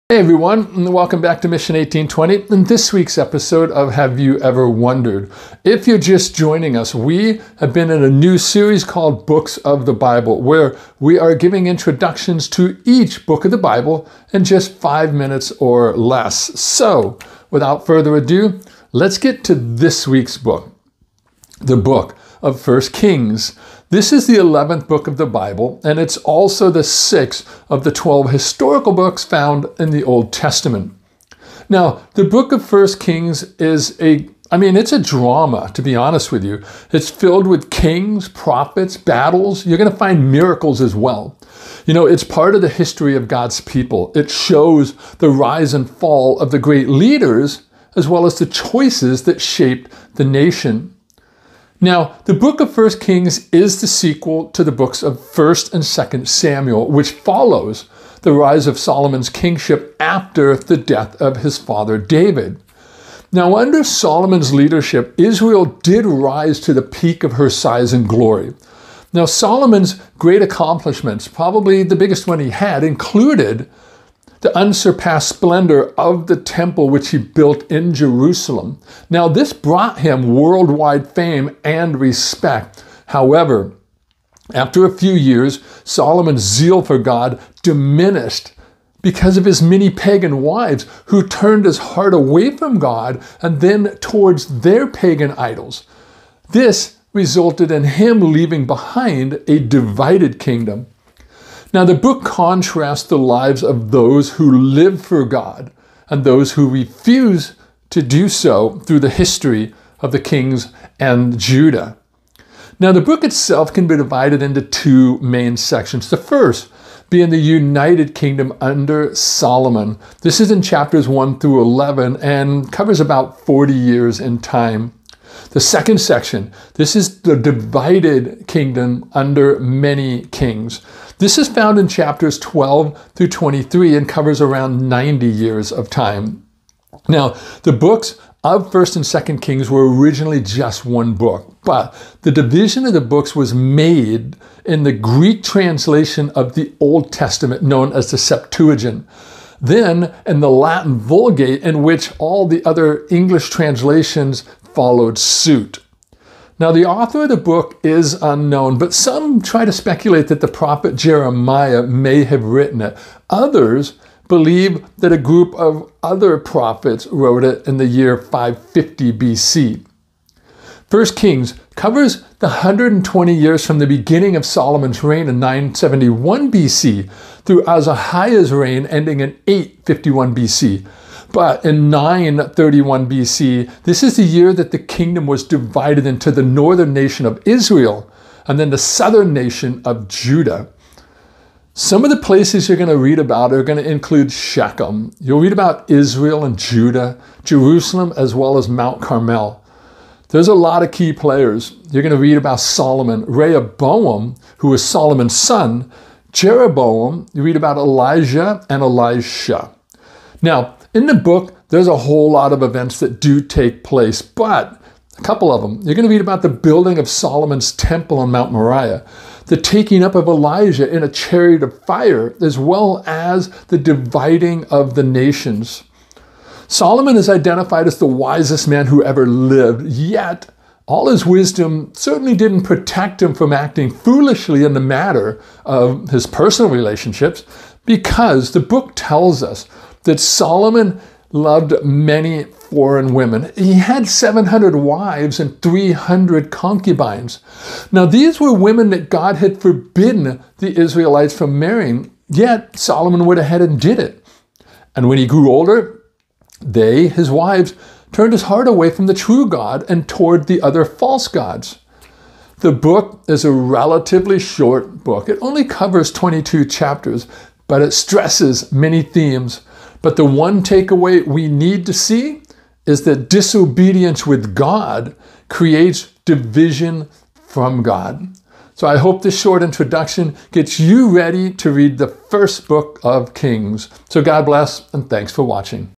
Books of the Bible: Brief Introduction to the Book of 1 Kings
In this week's episode of "Have you ever wondered?" we are in the 11th week of "Books of the Bible" and in the Book of 1 Kings.